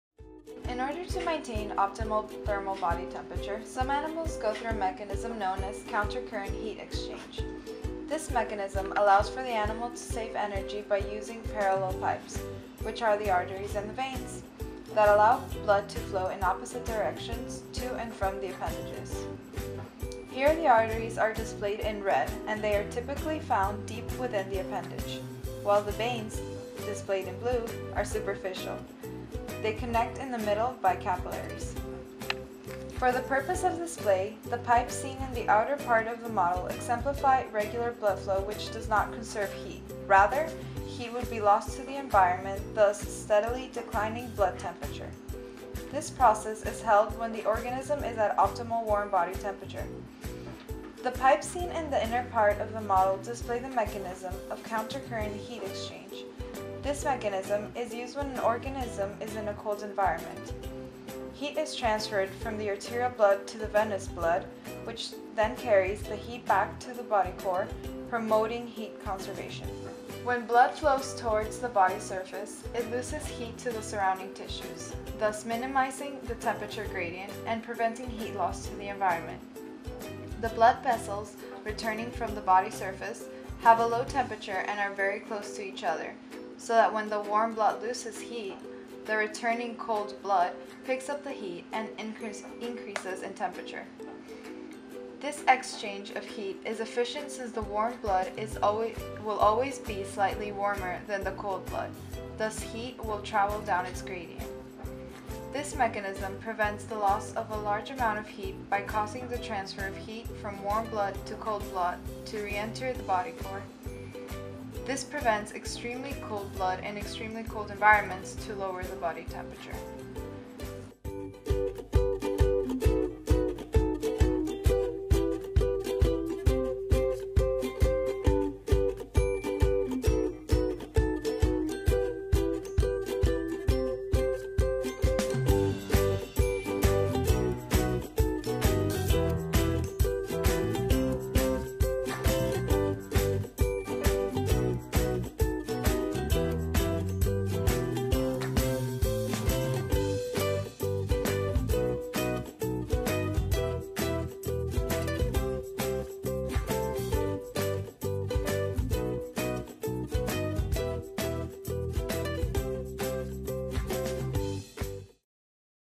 Countercurrent Heat Exchange
This is a video explaining the process of Countercurrent Heat Exchange using Thermochromatic Pigment.
Animal Physiology:
Talissa Canella-Szabo
Martina Lopez
Courtney Mondo